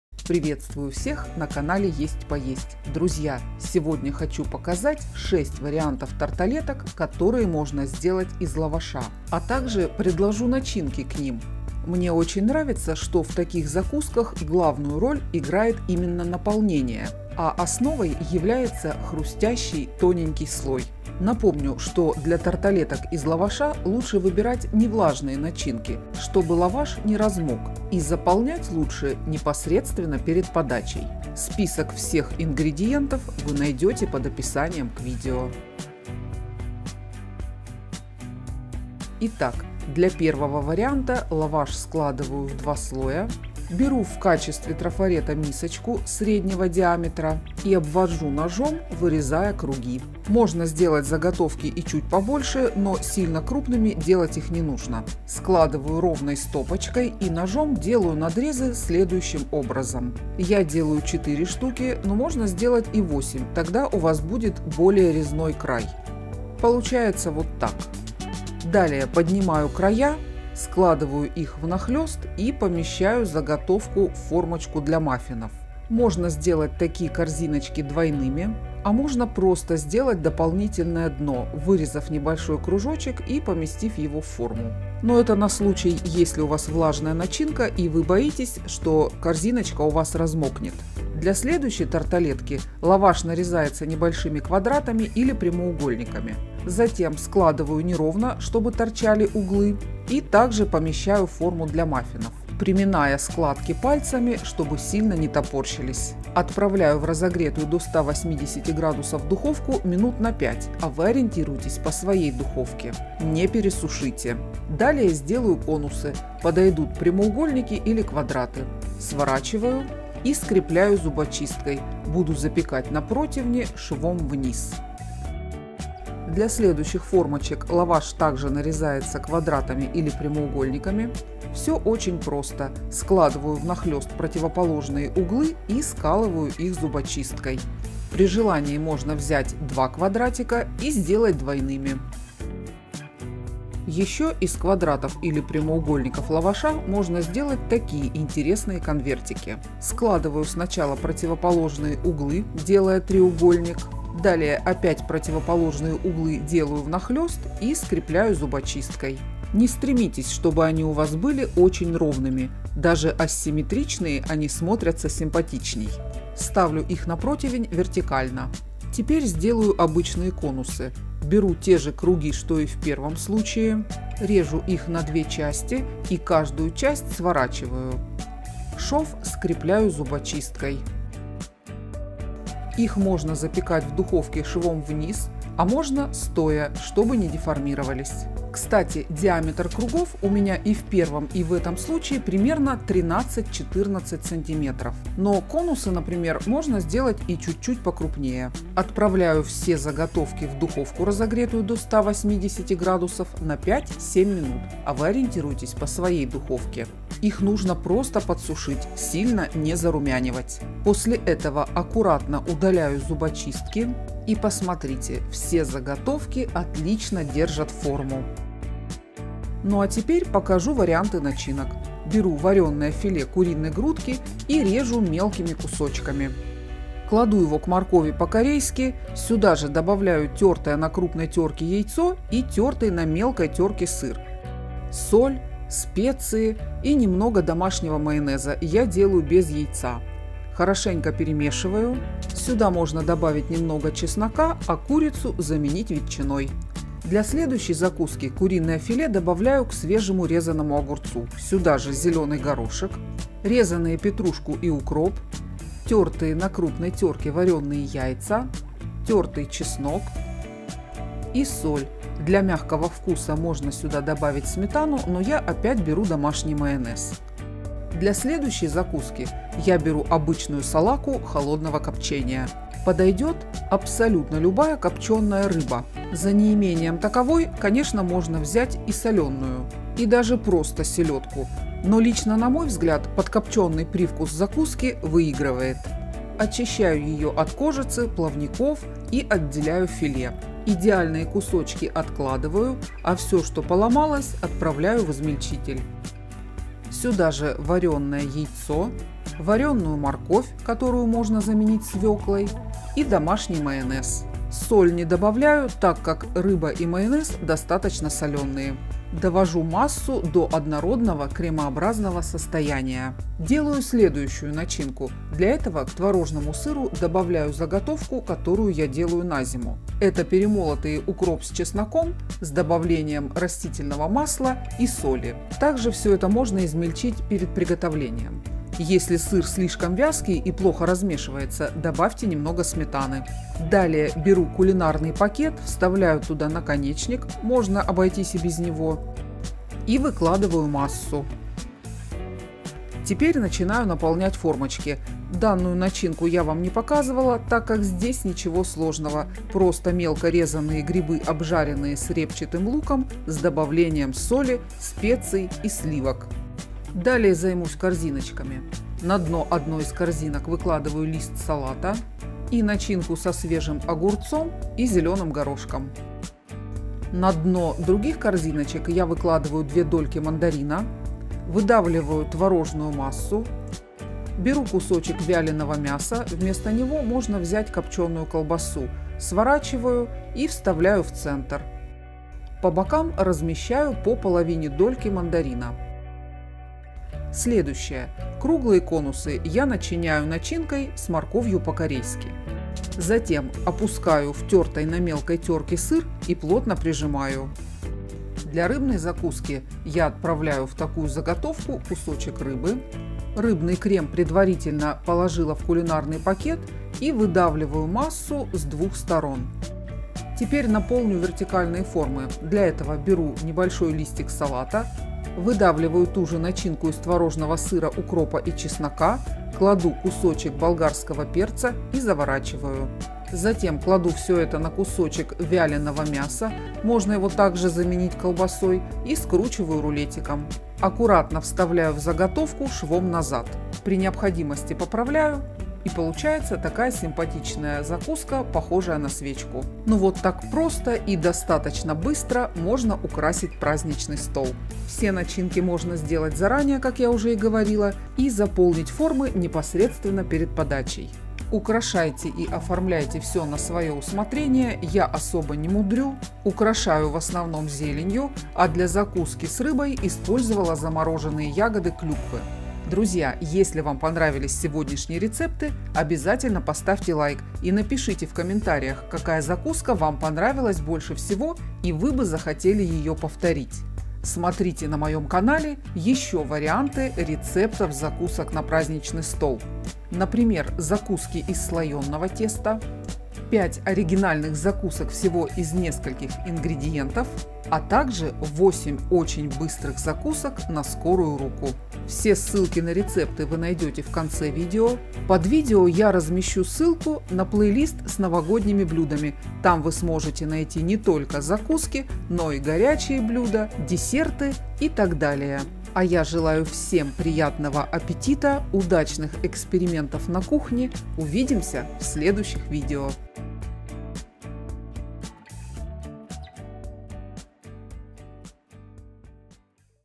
6 крутых тарталеток из лаваша с начинками.
Предлагаю шесть вариантов тарталеток, которые можно сделать из лаваша. В таких закусках главную роль играет начинка, а тесто служит лишь тонкой хрустящей основой. Наполнение и его количество можно выбирать по собственному желанию, поэтому, в предложенных мной начинках  не указываю точное количество ингредиентов. Помните, что для таких закусок подходят не излишне влажные начинки, чтобы избежать  размокания лаваша.

ИНГРЕДИЕНТЫ ДЛЯ НАЧИНОК:

1. грибы,
репчатый лук,
сливки или сметана,
растительное масло для жарки,
соль,
специи

2. огурец свежий,
филе куриной грудки отварное,
зелёный горошек,
варёное яйцо,
зелень,
домашний майонез или сметана,
листья салата

3. морковь по-корейски,
филе куриной грудки,
тёртый сыр,
варёное яйцо,
соль,
домашний майонез,
чеснок по желанию

4. яйцо варёное,
морковь варёная,
рыба копчёная,
домашний майонез

5. творожный сыр,
укроп,
чеснок,
мясо вяленое или копчёное,
мандарин,
соль

6. творожный сыр,
укроп,
чеснок,
копчёное или вяленое мясо,
листья салата,
перец болгарский,
соль